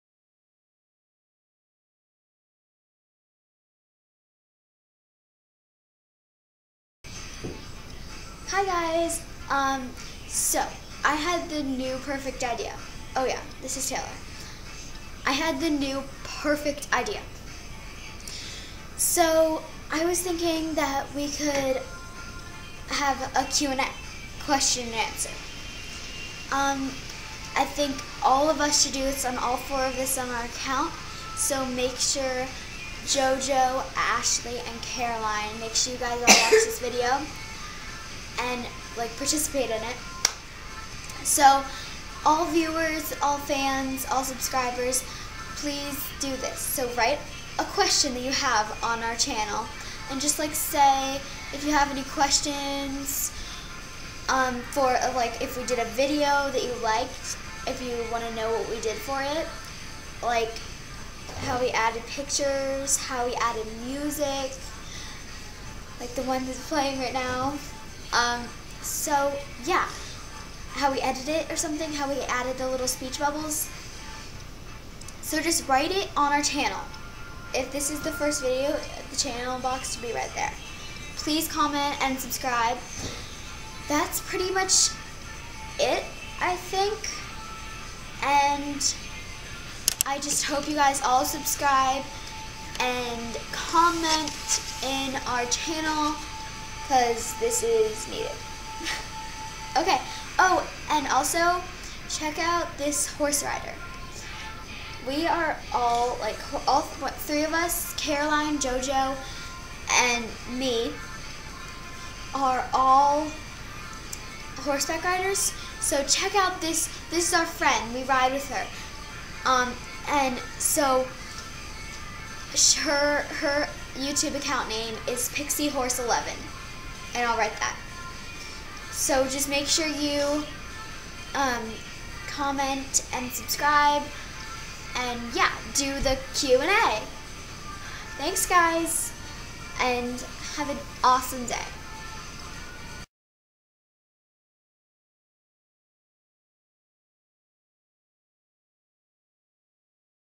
NEW IDEA!! WATCH NOW!!! :)
SO JUST WANTED ALL OF YOU GUYS TO ASK QUESTION AND MY FRIENDS AND I WILL ANSWER!!!  BY TAYLOR AKN